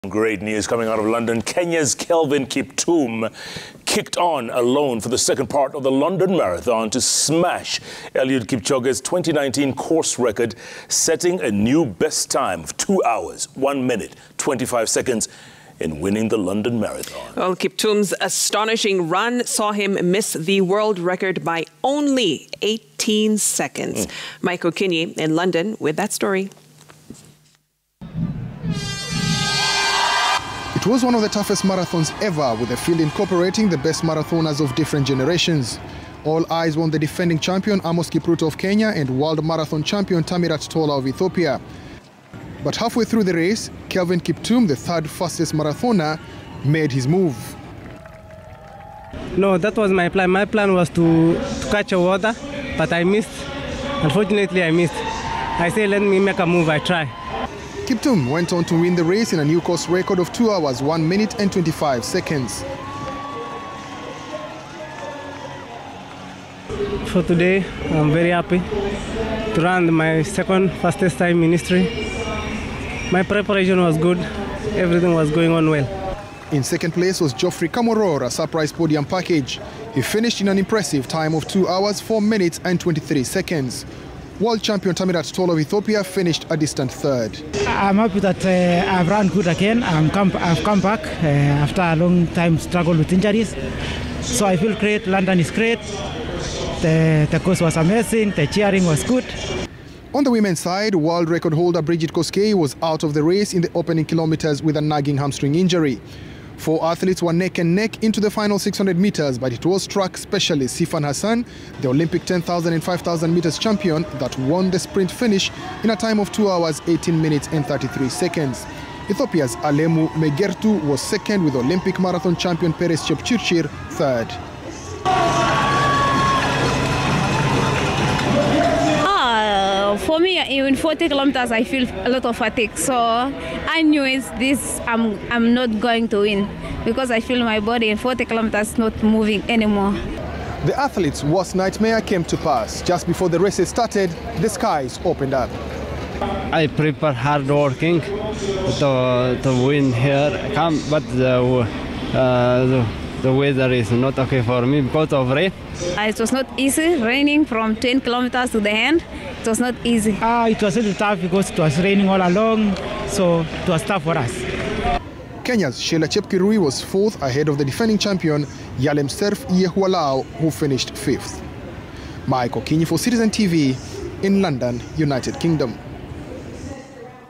Kelvin Kiptum wins London marathon on debut
Kenya’s Kelvin Kiptum kicked on alone for the second part of the London marathon to smash Eliud Kipchoge’s 2019 course record, setting a new best time of 2 hours one minute and 25 seconds in winning the London marathon. Kiptum’s astonishing run saw him miss the world record by only 18 seconds.